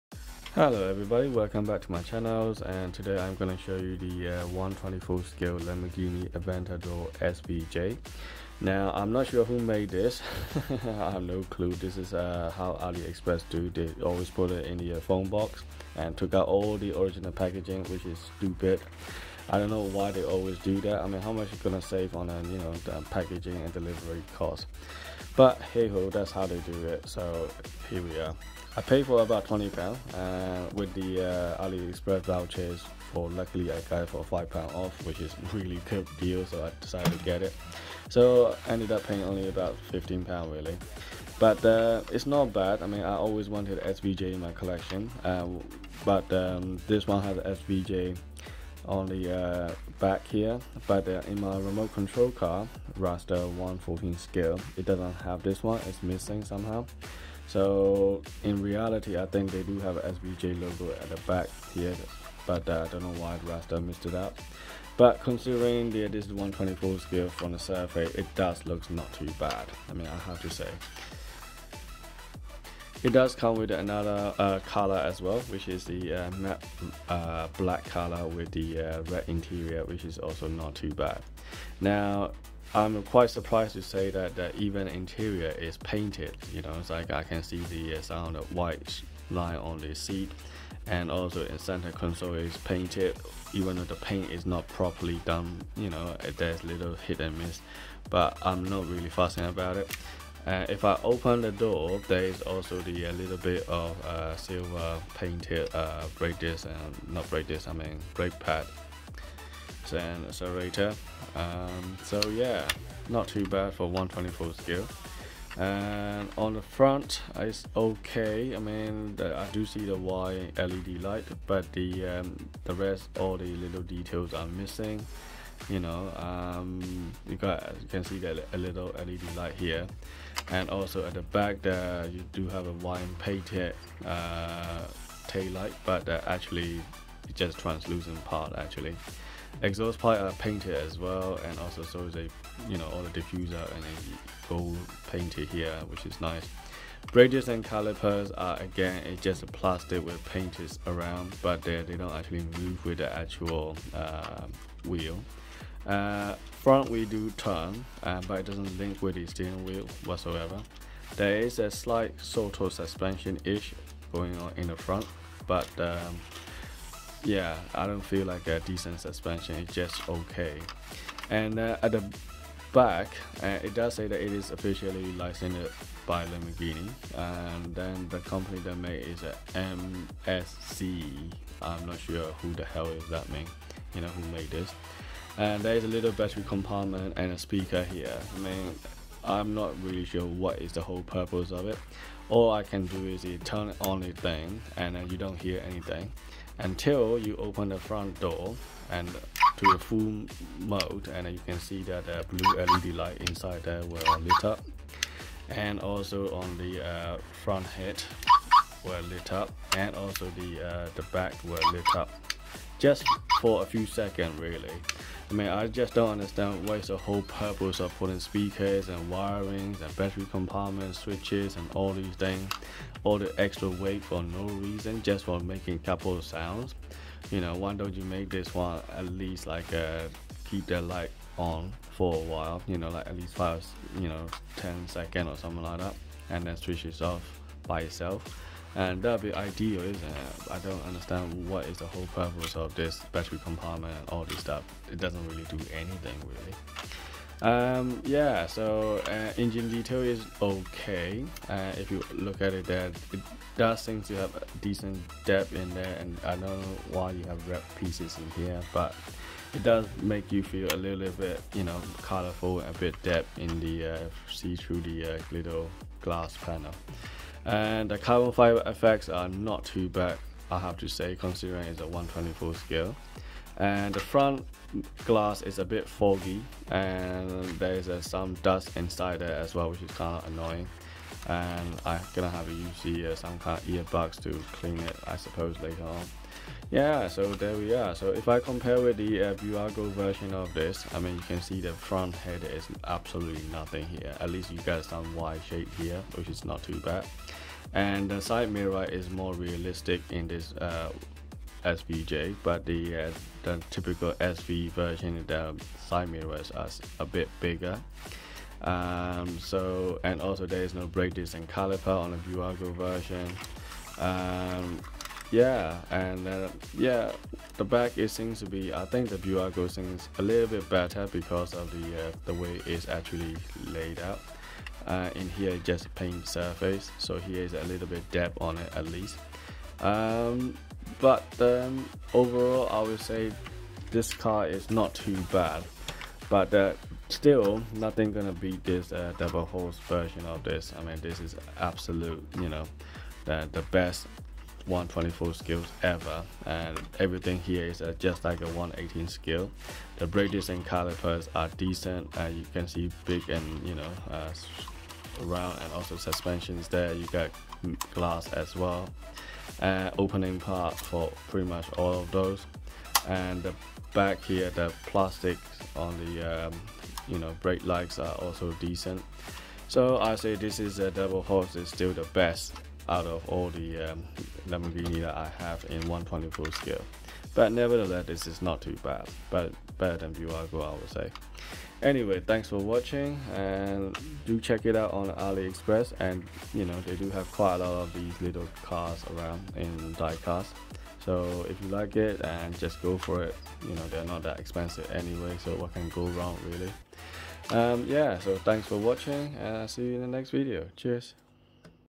Who made this model car? Lamborghini Aventador SVJ - 1/24 Diecast with Light and Sound - Full Review
Pros
Nice consistent paint all around
Reasonable details inside and outside
LED on both front and back
Interior has painted seats and blue LED
3 opening parts (2 doors and engine)
Front wheels turn and go back to straight position with ease

Cons
Brake discs does not rotate with the wheels
Light and Sound effect is kind of pointless since only work when you open the doors.
No window or suspension but then most of them don't come in 1/24 scale.

Got it for about £15 - 20 from Ali Express. Not too bad details in term of scale and I would say fairly reasonable. Just don't see any purpose or use for having battery compartment, speakers, switch and all this wiring. Not to mention added weight to the model for just a couple of second for light and sound effect.